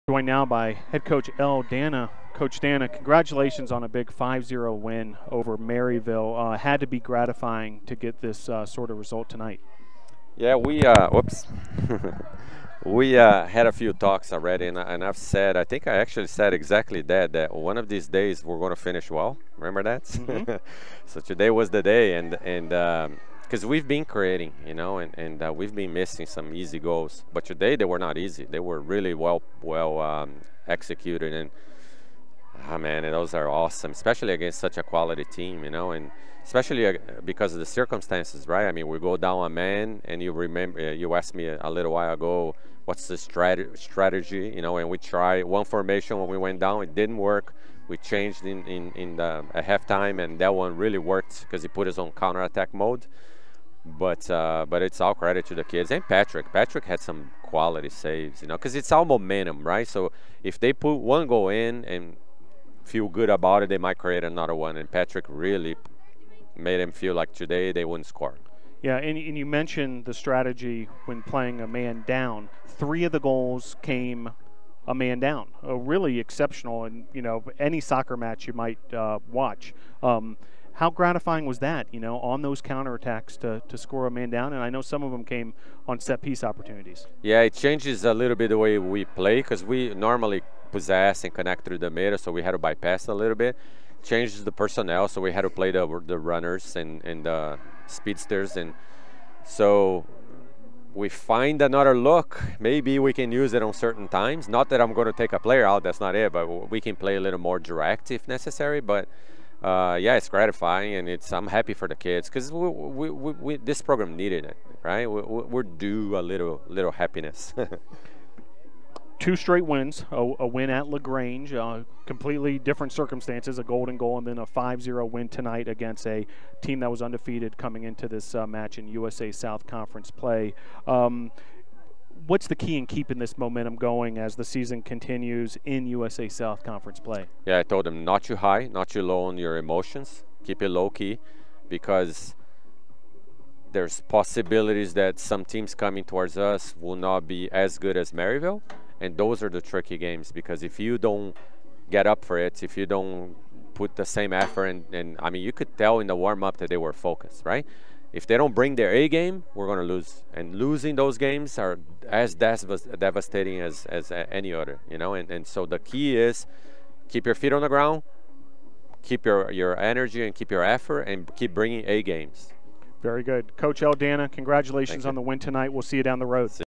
Men's Soccer: Interview with Head Coach Helio "L" D'Anna following big 5-0 win vs. Maryville
Brevard College Tornados men's soccer Head Coach Helio "L" D'Anna's interview following BC's big 5-0 USA South Conference victory over Maryville College at Ives-Lemel Family Field on Wednesday, October 2.